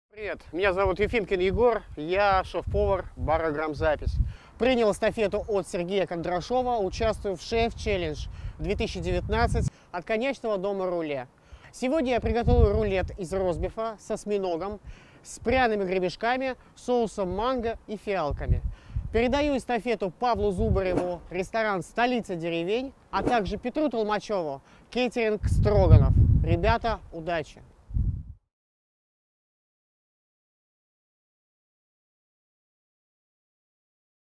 Егор Ефимкин в Roullet Chef Challenge
Шеф-повар бара «Граммзапись» Егор Ефимкин принял эстафету от Сергея Кондрашова и приготовил рулет из ростбифа с осьминогом, пряными гребешками, соусом Манго и фиалками.
Передал эстафету Павлу Зубареву – ресторан «Столица деревень» и Петру Толмачёву – кейтеринг Stroganoff.
Удачи!